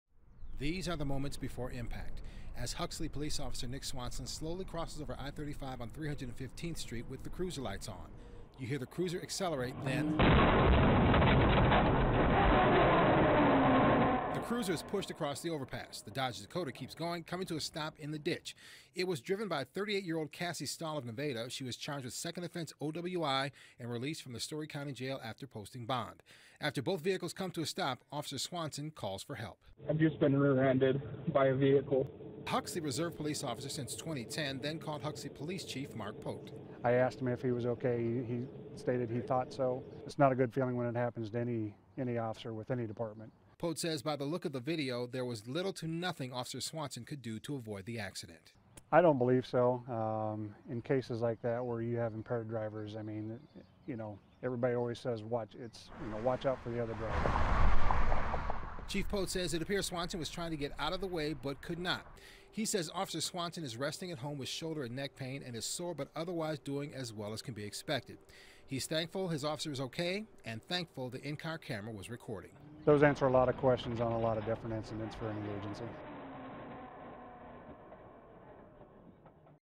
Video: Iowa police cruiser rear-ended by drunk driver
Huxley police officer Nick Swanson was violently rear-ended by a drunk driver on Thursday. Reporter Marcus McIntosh shows us the dramatic video, which was all caught on the cruiser's dash cam.